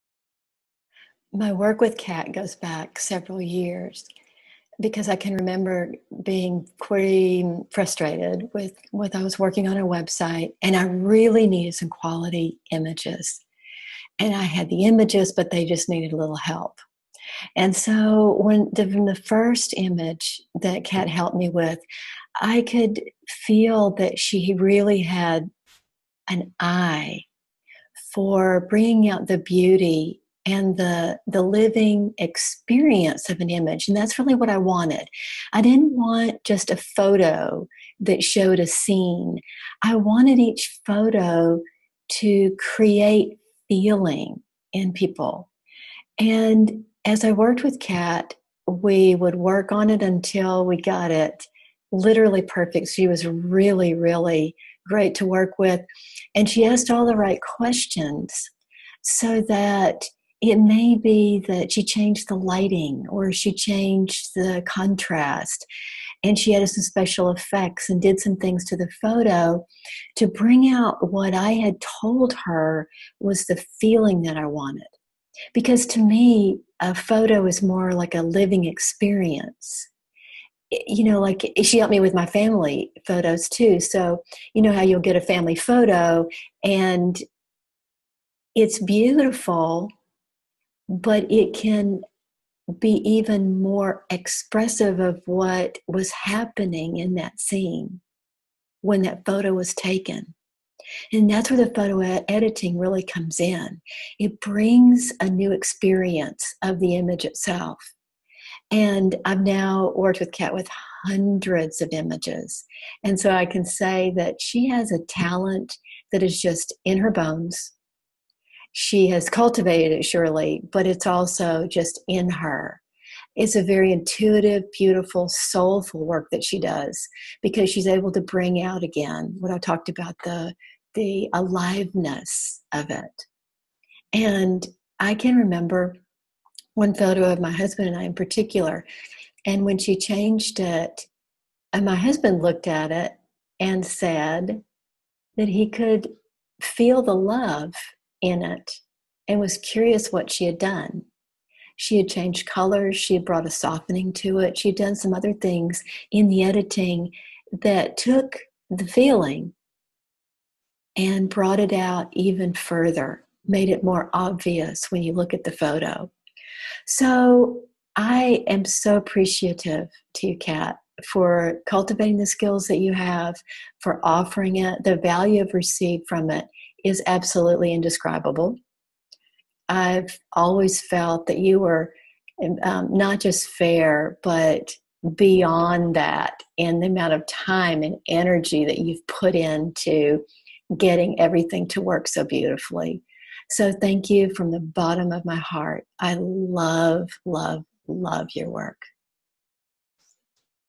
Sweetest testimonial on KatPhotoMagic's Photo Retouching
A sweet testimonial from a client turned friend after many years of working together editing hundreds of her photos.

if you love to photograph your family using your digital camera and would LOVE to make beautiful prints and gifts with them.

Katherine Wheeler
Family Photo Retouching services